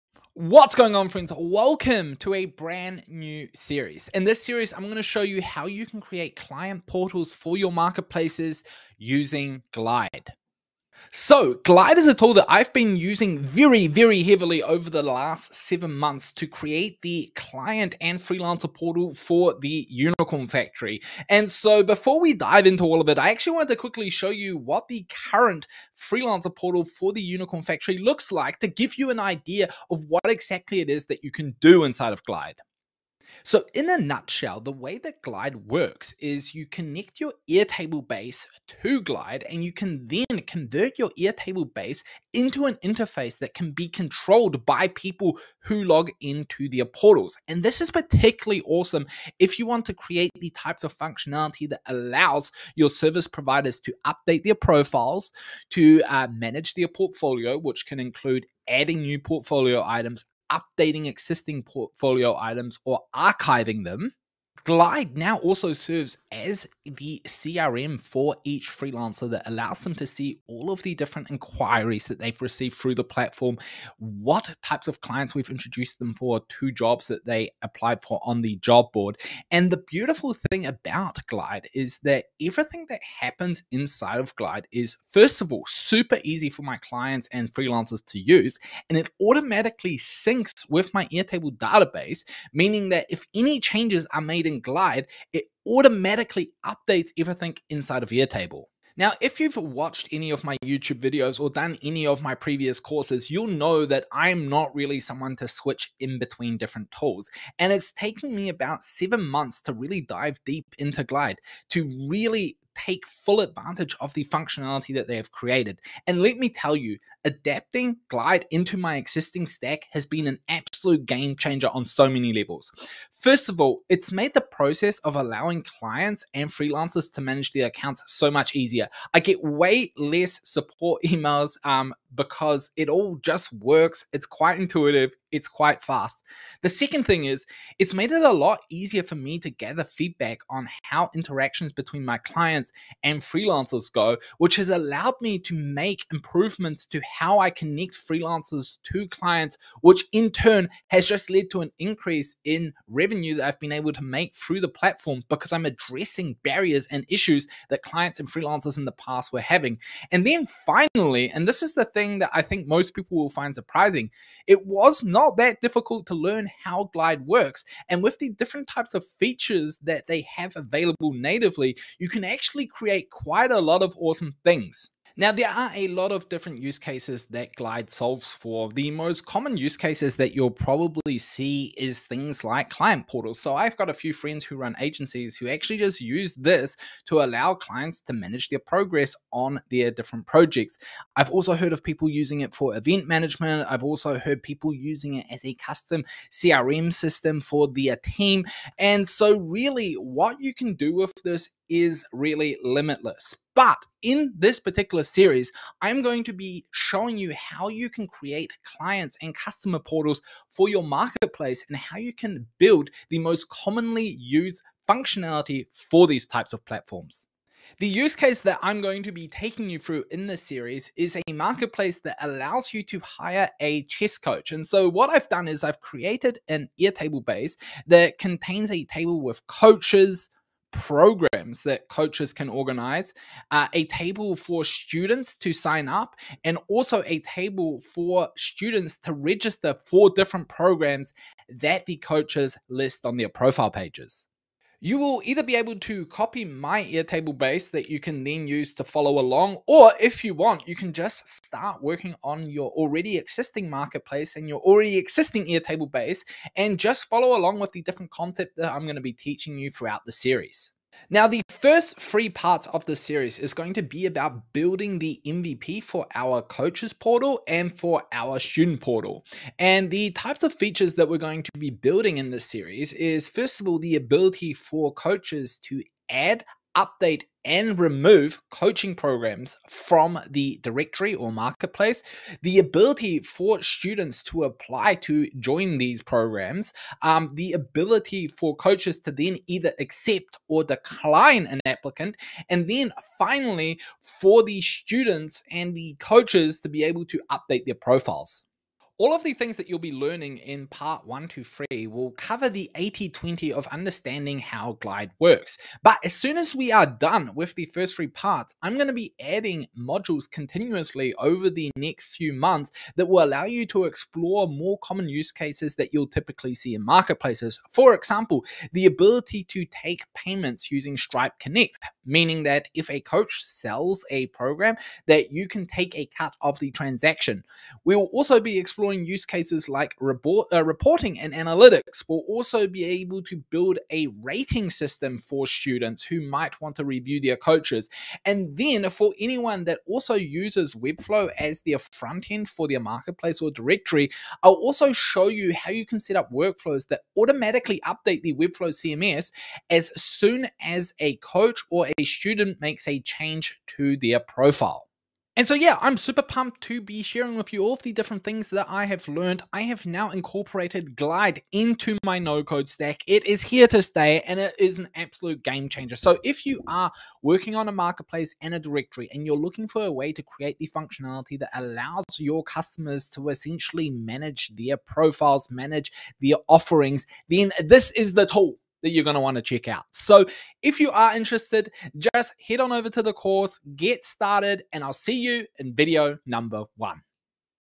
Setting Up Client Portals in Glide for Your Two-Sided Marketplace | Course Preview
Welcome to my brand new course on how to build customer portals for two-sided marketplaces in Glide! Glide is an incredible tool that can revolutionize your business, as I've experienced firsthand while working on the Unicorn Factory. Over the past seven months, I've delved deep into this tool, using it to create a functional and user-friendly portal for clients and freelancers.

In this course, I'll give you a detailed walkthrough of how you can create similar client portals for your own marketplace. We'll connect your Airtable base to Glide and transform it into an interactive interface that your clients can easily navigate and control.

Throughout the series, you'll learn how to allow your clients to update their profiles, manage their portfolio, and interact with your marketplace effortlessly. The changes made in Glide sync automatically with your Airtable database, simplifying data management and customer service.

In this course, I'll walk you through the process of building a marketplace to hire chess coaches, demonstrating the functionality needed in a two-sided marketplace. The first three parts of this series will cover the MVP for our coaching and student portals, including updating profiles and managing program applications.

However, this is just the beginning. The course will continuously evolve over the next few months, with new modules exploring more common use cases that you'll typically see in marketplaces. We'll cover Stripe Connect for payment processing, reporting and analytics, rating systems, and more.

Whether you're new to Glide or you've already incorporated it into your No-code stack, this course is perfect for those looking to optimize their marketplace or directory. So, don't wait! Get started and enhance your Glide knowledge.

GET IN TOUCH:
If you have any specific questions about how to do things with any of the tools please leave a comment on the video or on the channel.